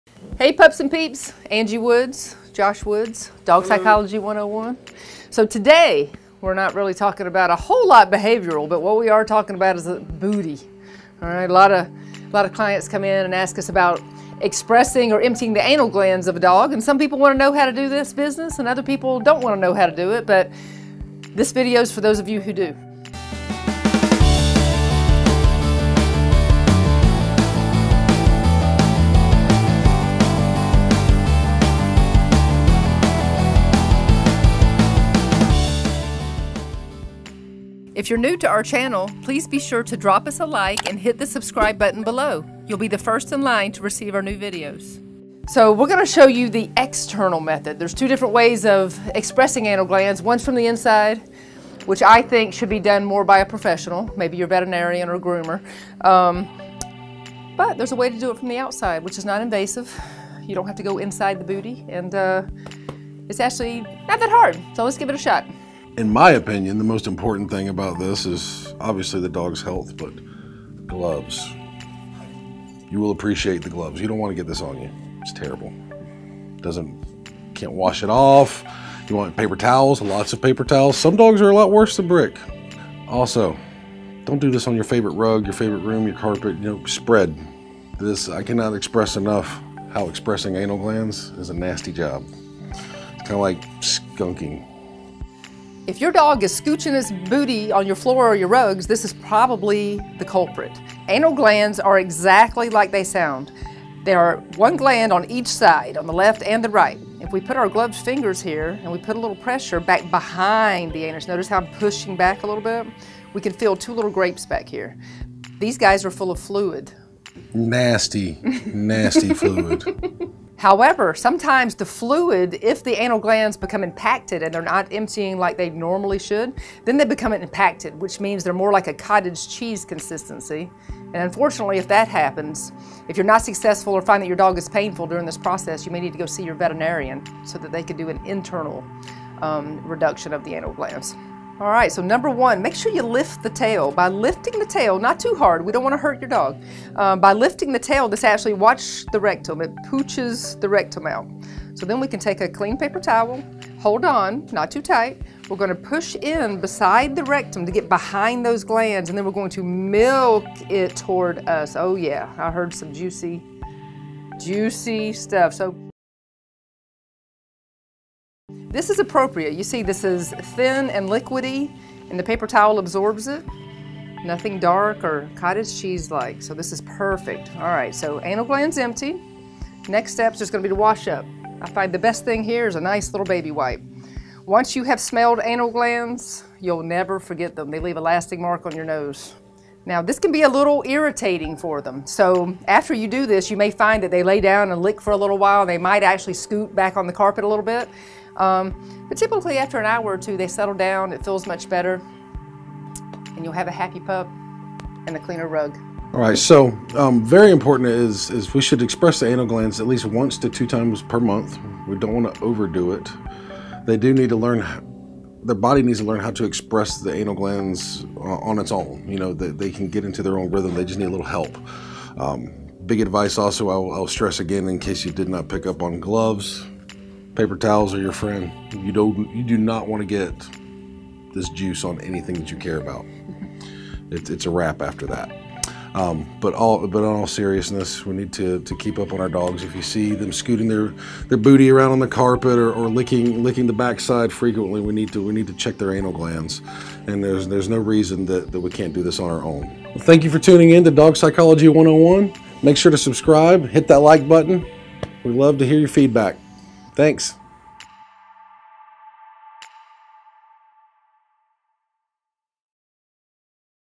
How to Express Your Dog's Anal Glands at Home | DIY Anal Gland Expression
How to Express Your Dog's Anal Glands at Home |

Do you ever find your dog scooting its butt on your carpet? Maybe they’re prone to chewing or licking on their back end? These behaviors are usually caused by their anal glands. If the dog’s body doesn’t efficiently express the anal glands on its own, they can become impacted and cause lots of irritation for your dog. Anal glands not being expressed can become a serious health issue in the long run. In this video, Angie and Josh demonstrate how to express your dog’s anal glands at home. This is something that most people can do without an expensive trip to the vet. This video will teach you how to express your dog’s anal glands at home with confidence.